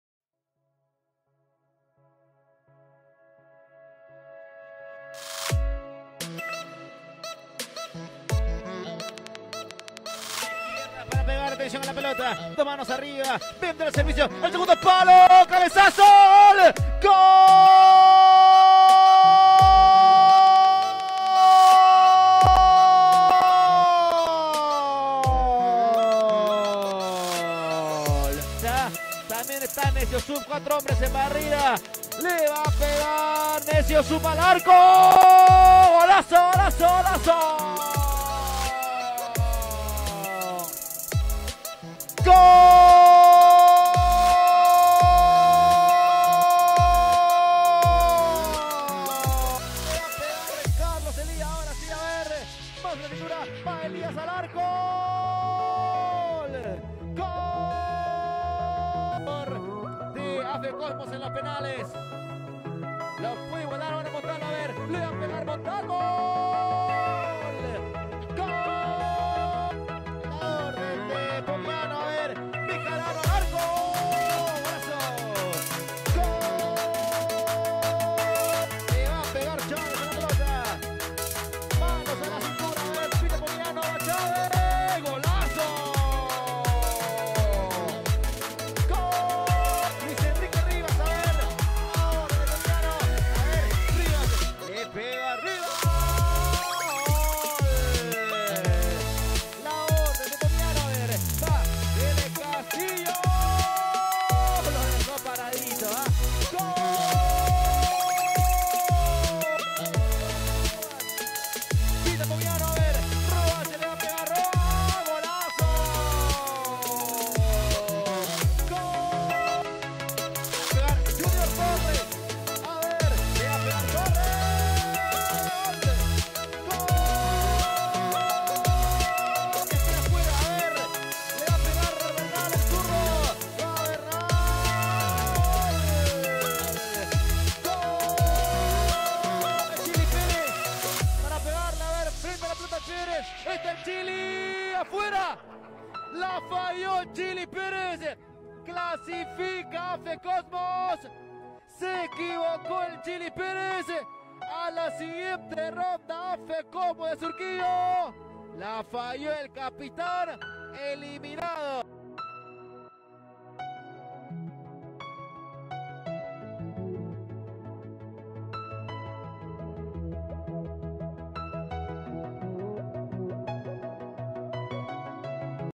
RESUMEN| COPA PERÚ 2022 INTERLIGAS FASE 3 - AFE COSMOS (SURQUILLO) VS DREAM TEAM (SJL)
INTERLIGAS 2022 FASE 3 | AFE COSMOS VS DREAM TEAM
AFE Cosmos, de Surquillo, venció a Dream Team, de San Juan de Lurigancho, en un duro partido. Fue empate a 1 en los 90' y todo se definió desde el punto de penal. INFARTANTE DEFINICIÓN.